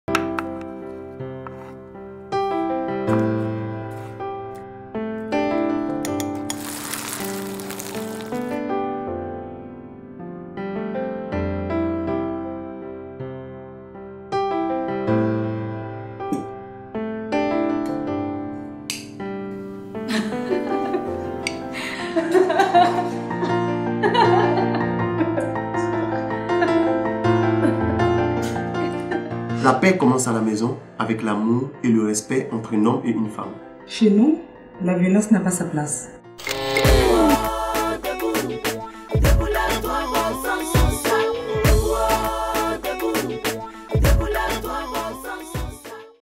Chez Nous, La Violence n'a pas sa place! (In our home, violence has no place!)
One of four spots for the Break the Silence (Brisons le Silence!), a social marketing campaign to prevent violence against women and promote gender equity in Cote d'Ivoire, Africa. Features football (soccer) star Kolo Tore and his wife.  Message is to promote gender equality.